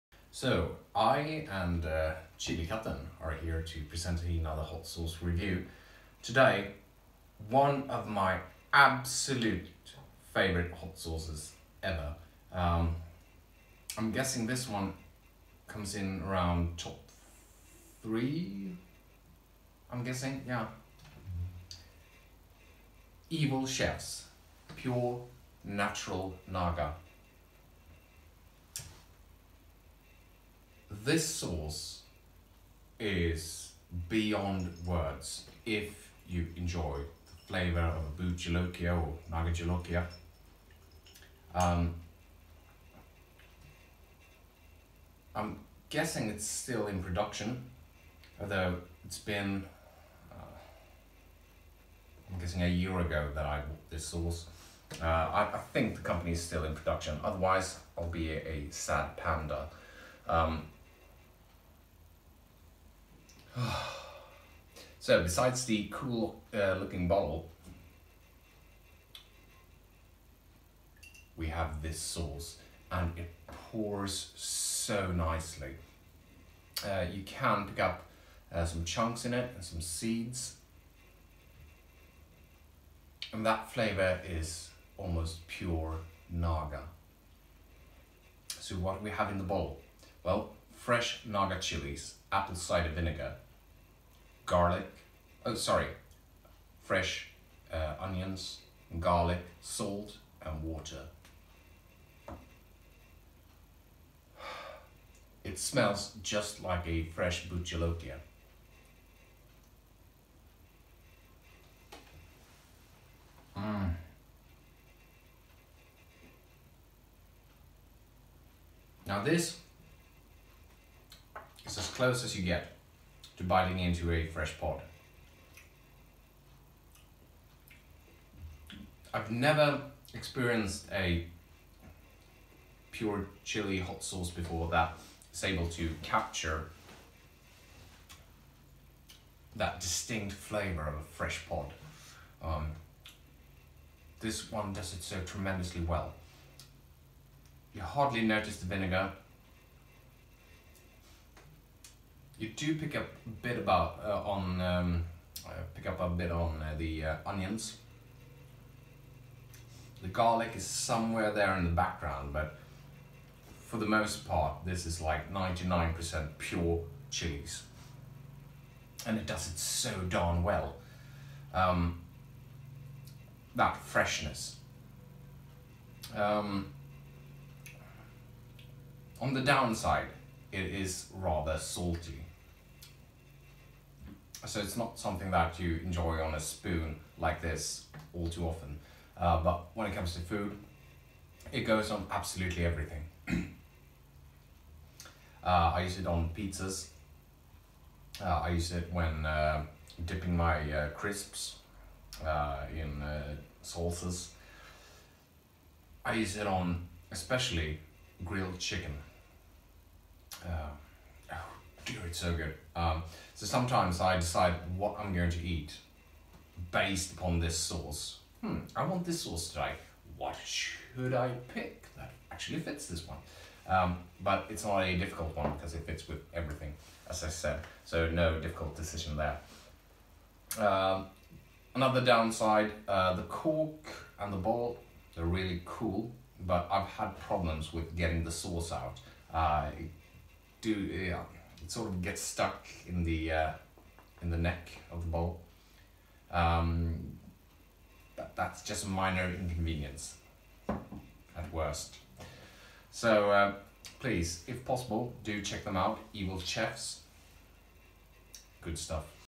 Hot Sauce Review: Evil Chef's No1 Pure 100% Evil Natural Naga Hot Sauce
One of my absolute favourites! Pure Naga flavour all the way! I've had their Limited Harvest sauce in 2013 which was even better and came in a wooden box. yet, alas, that one has already been consumed. In addition, their chili-salt is pretty darn good as well. Great company is great.

Evil Chef's? Shouldn't it be Evil Chefs'? I will excuse their grammar for the sake of their awesomeness. :-)

Här hittar du allt för att stilla din mun gällande hetta. :-) Och nej, jag är inte sponsrad av någon. Jag ger enbart ut sköna tips. :-) Kolla gärna in